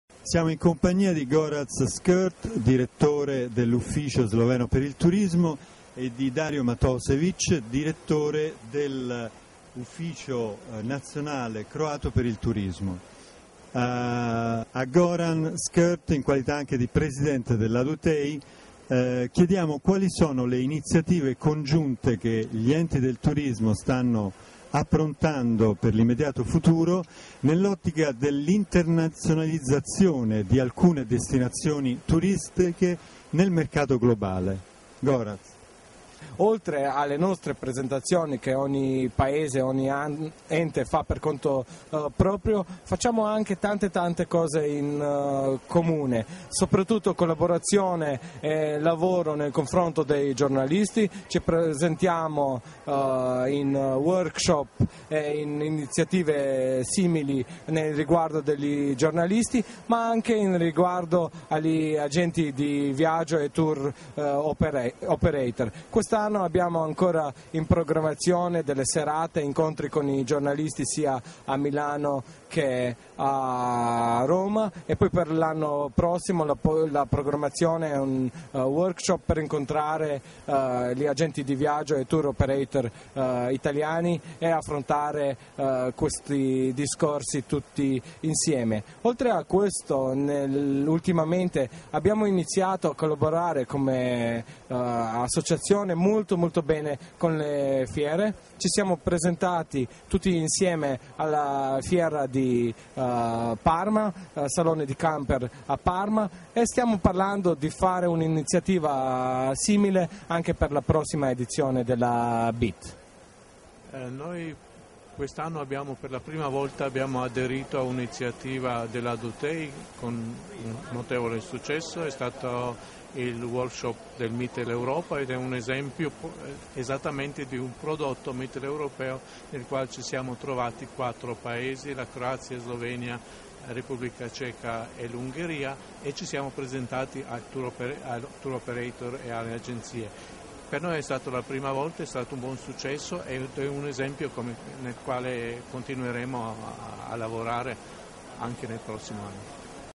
Turismo Sloveno - Turismo Croato
Ai nostri microfoni Gorazd Skrt, direttore per l'Italia dell'Ufficio Sloveno per il Turismo, e Dario Matosevic, direttore per l'Italia dell'Ufficio Nazionale Croato per il Turismo. Si parla delle iniziative congiunte che i due enti del turismo stanno approntando per il mercato globale.